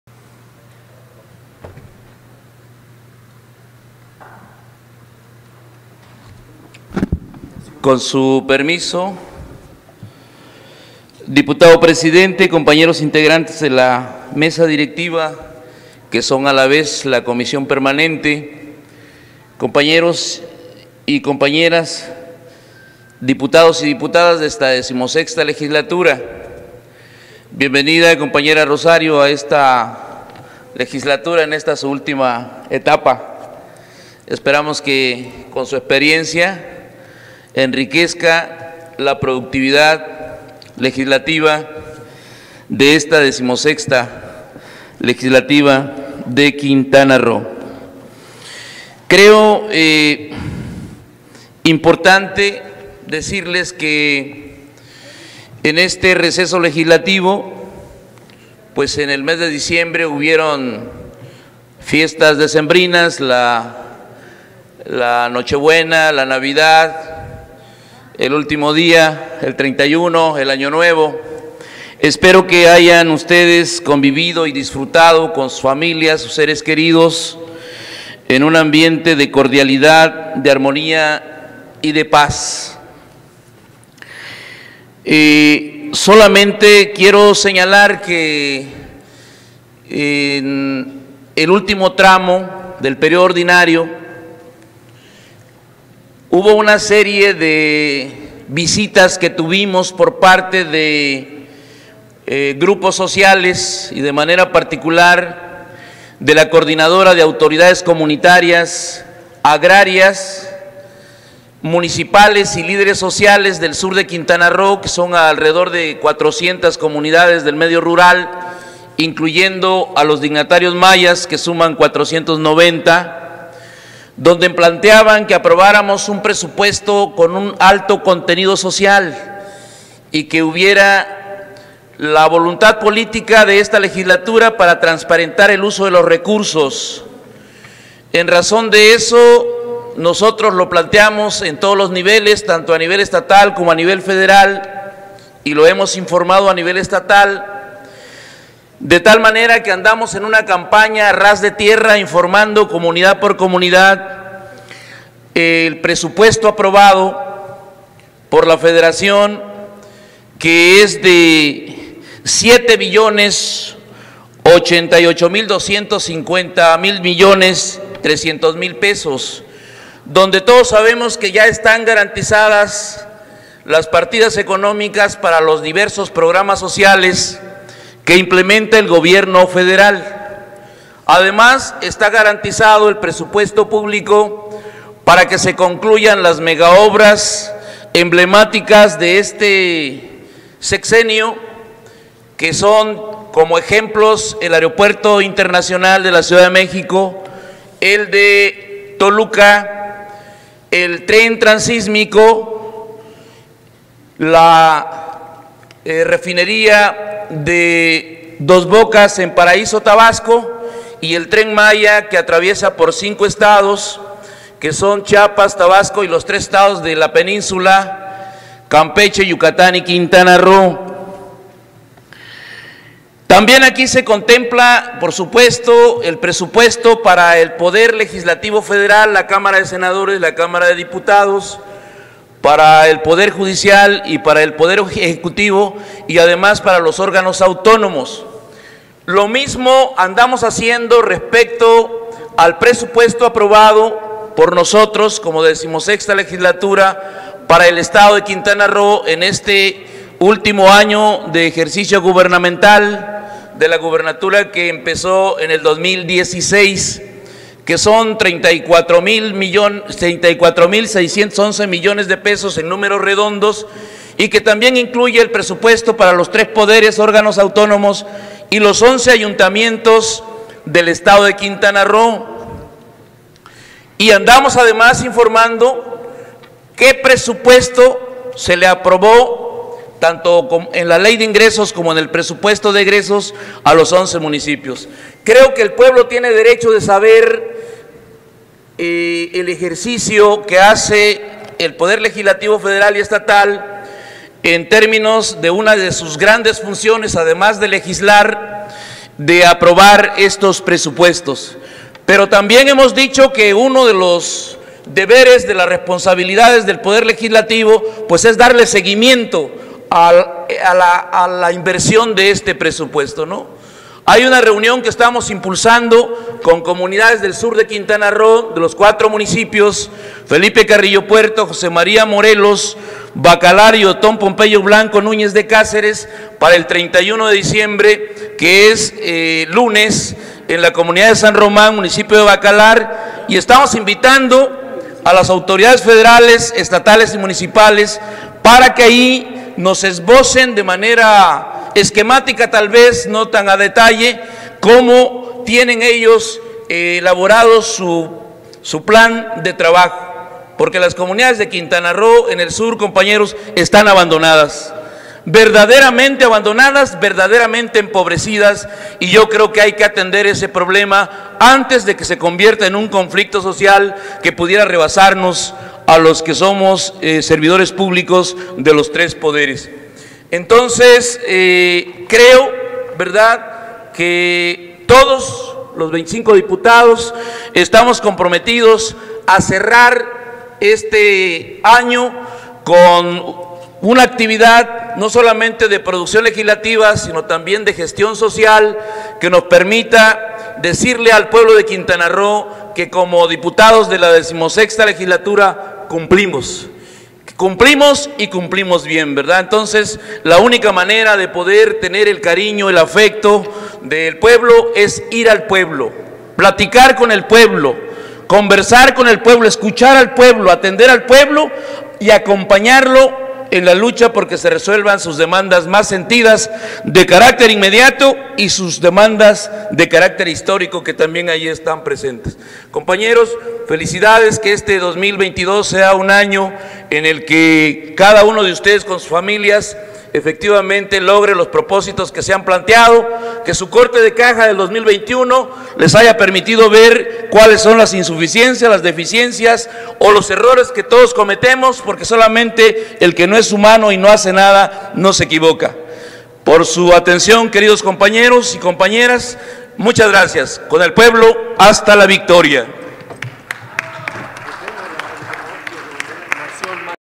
Intervención del Dip. Hernán Villatoro Barrios. PT (19/Ene/2022)
Intervención del Diputado Hernán Villatoro Barrios, Presidente de la Comisión de Trabajo y Previsión Social de la XVI Legislatura, en el  Primer Período Extraordinario de Sesiones del Tercer Año de Ejercicio Constitucional.